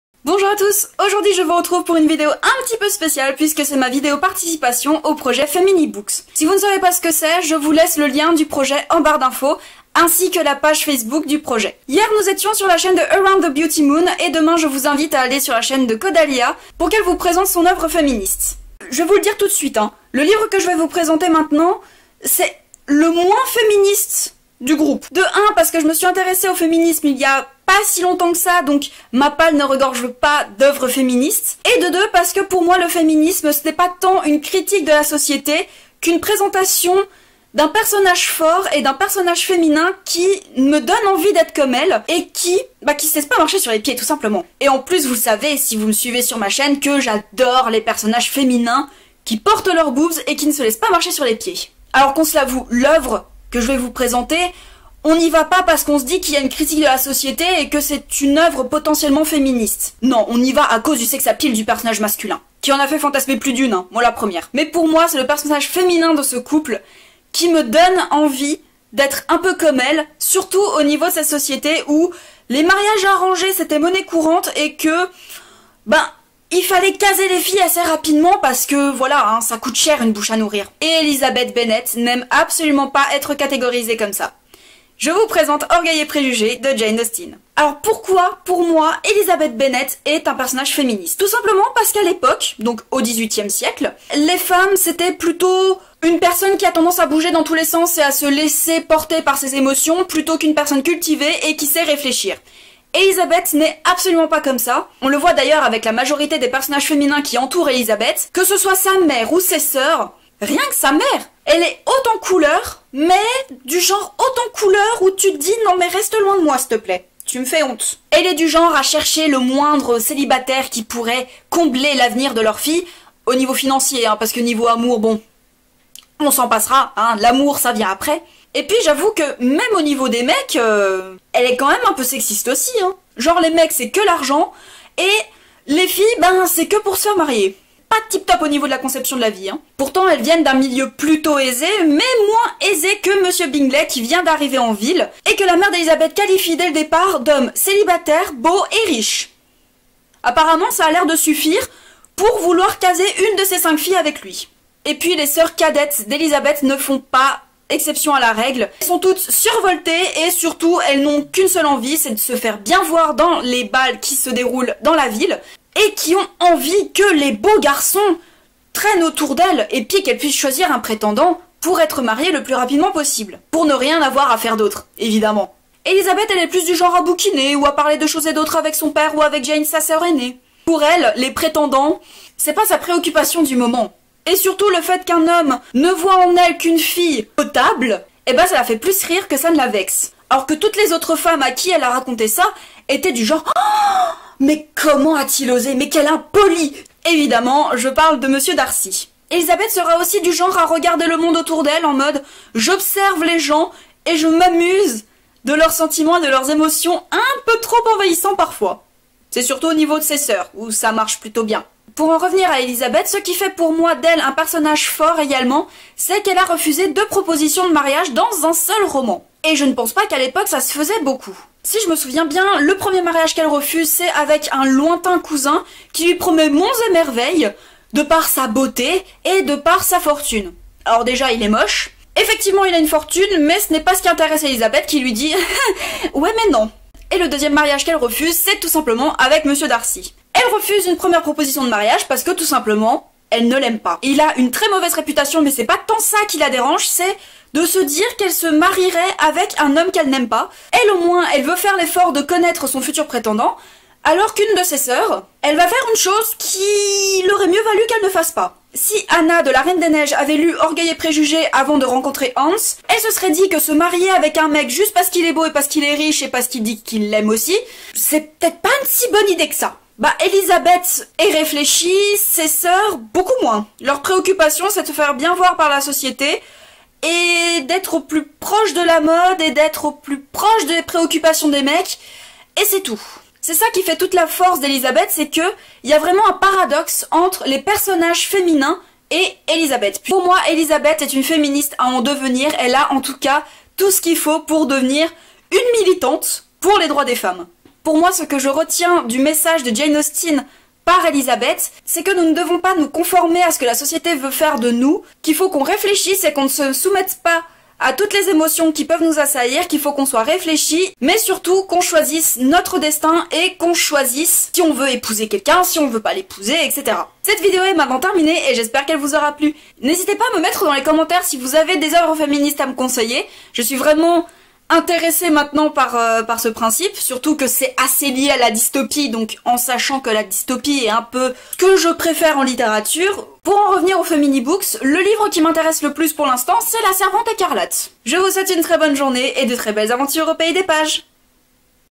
Projet Femini Books #20 | Lizzy, une féministe en devenir
Aujourd'hui je vous retrouve pour ma vidéo participative pour le projet Femini-Books, qui est un projet réalisé par Opalyne, qui se déroule tout le long du mois de mars et qui invite des Booktubeurs à parler du Féminisme dans la littérature. Evidemment je n'ai pas pu résister et j'ai répondu présente à l'invitation. Je vous présente donc le livre Orgueil et Préjugés de Jane Austen, qui contient pour moi un personnage féminin qui pourrait être une féministe de son temps.

J'espère que cette vidéo vous aura plu et qu'elle vous a fait découvrir Orgueil et Préjugés sous un autre jour que celui qu'il est mit habituellement (surtout depuis Le Journal de Bridget Jones...), mais qu'elle vous aura aussi permis de découvrir le super projet créé par Opalyne.

Je vous retrouve très vite pour une prochaine vidéo !

Je filme avec : un appareil photo Nikon CoolPix S5200.
Je monte avec : Windows Movie Maker.
J'utilise les polices de DAFONT pour mes miniatures.